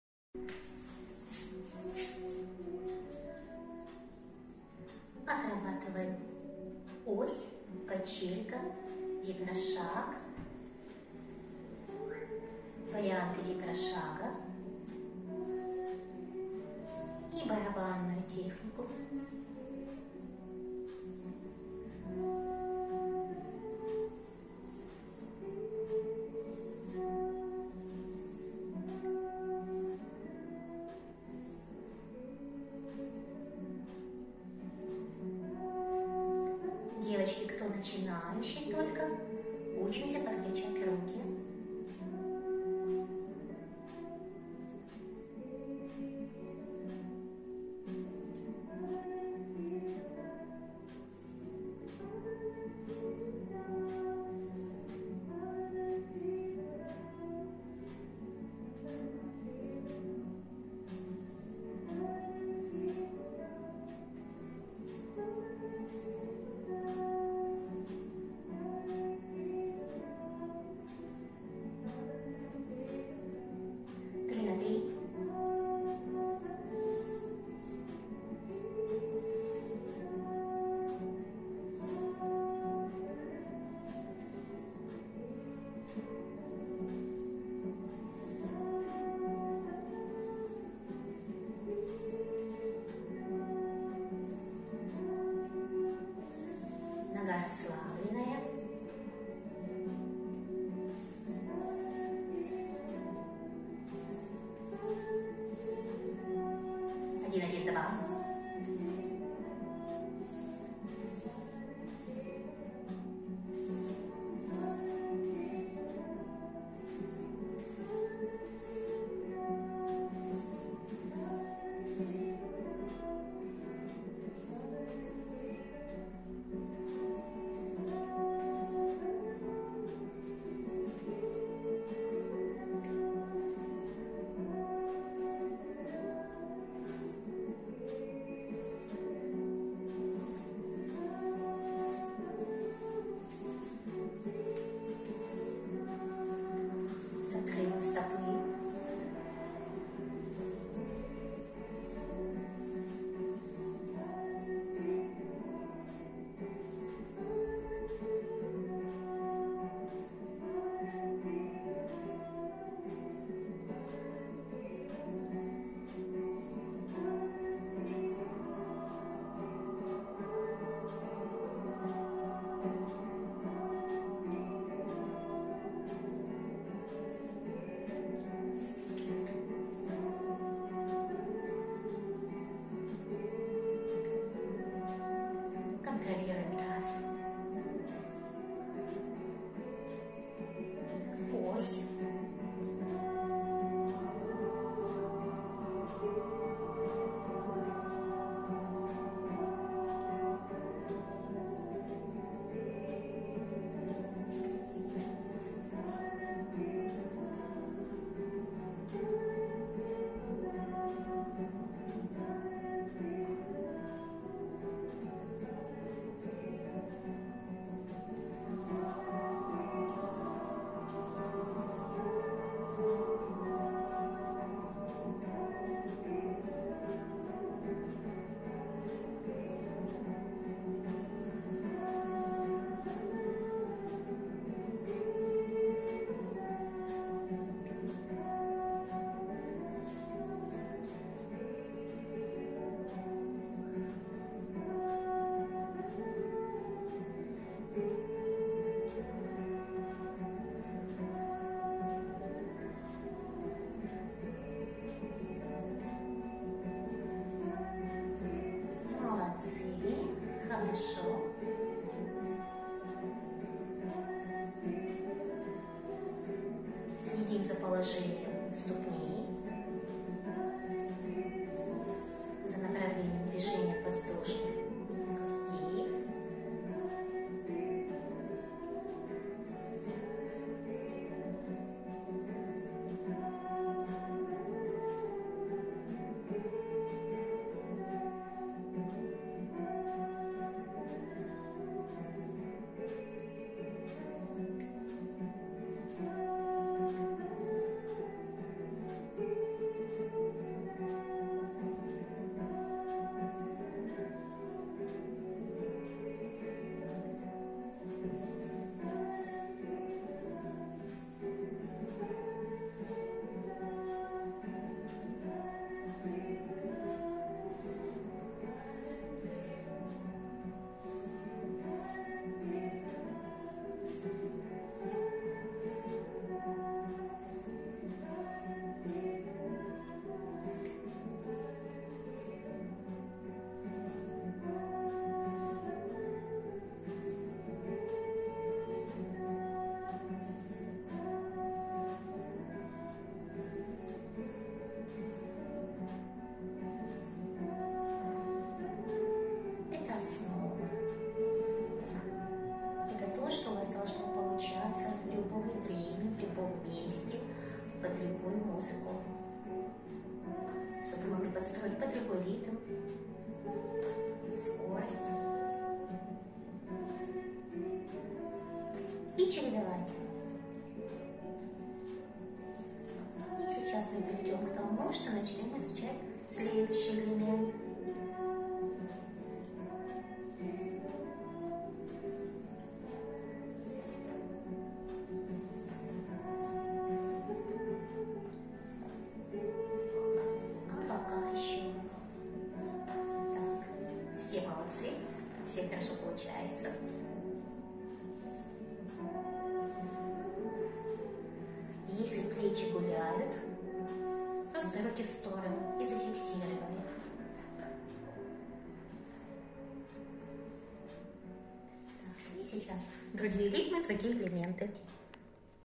Резонансные танцы. Лотосная техника. Базовые элементы. Практика
Включи и танцуй. Слушай музыку и чередуй техники под её ритм.
Встаньте путь выстраивания лучшей версии себя.
А я всегда, с удовольствием, вам в этом помогу.
и инстаграм  @19tatyana_shutova72